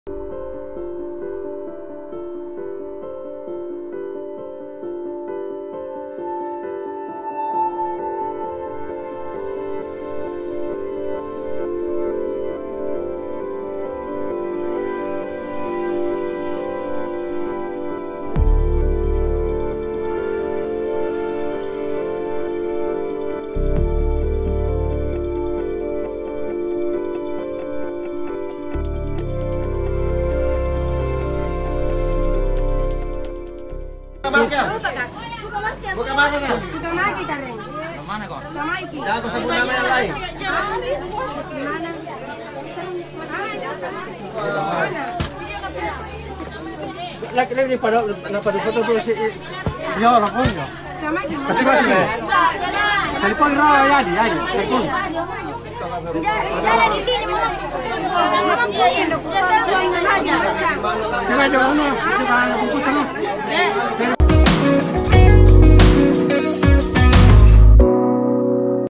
Pelaku Pencurian Emas 30 Gram di Pasar Karisa Jeneponto Berhasil Diamankan
Aksi pencurian emas 30 gram di Pasar Karisa, Jeneponto berhasil digagalkan oleh warga, Sabtu (2/7/2022)

Pelaku seorang perempuan. Dalam video terlihat warga yang mengintrogasi pelaku.

Warga yang kesal membuka jilbab yang pelaku.

Setelah diminta keterangan soal tempat tinggal, pelaku tersebut tidak menjawabnya dengan jujur.

Melainkan menyebutkan banyak nama kampung

Informasi terbaru, pelaku sudah diamankan oleh pihak yang berwajib.
Video terkini dan terbaru..